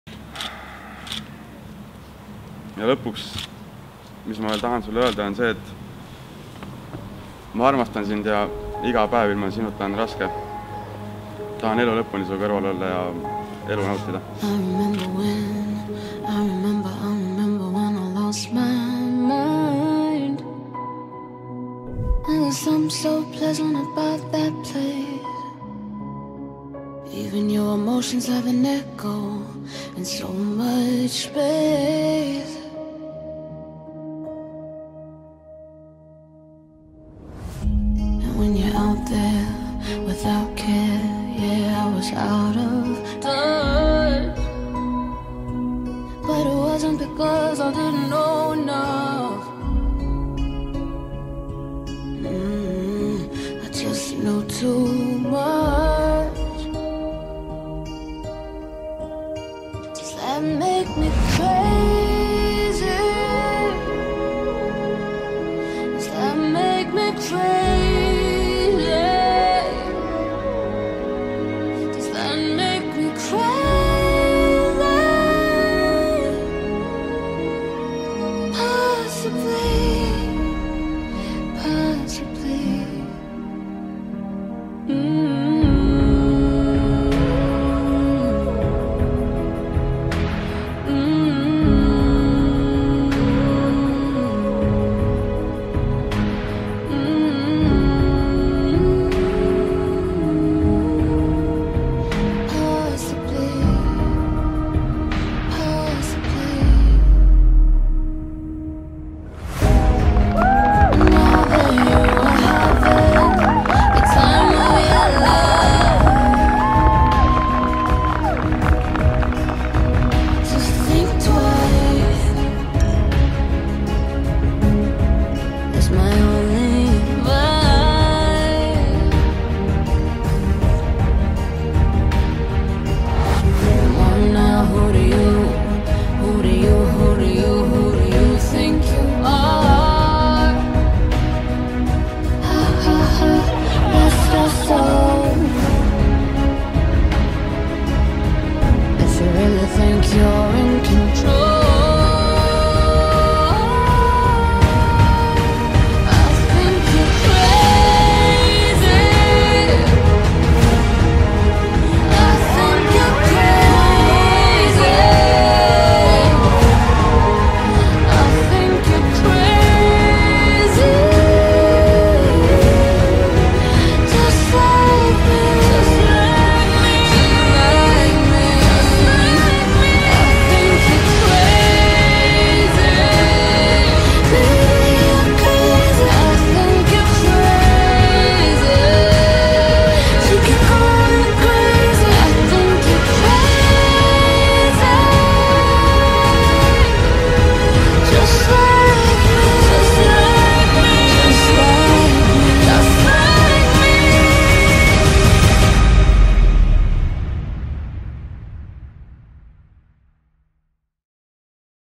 C&T Wedding video trailer
25. August 2018 took place C&T beautiful wedding which we had pleasure to capture. It was really nice to see that two beautiful human beings have found their soulmates and want to spend rest of their life together.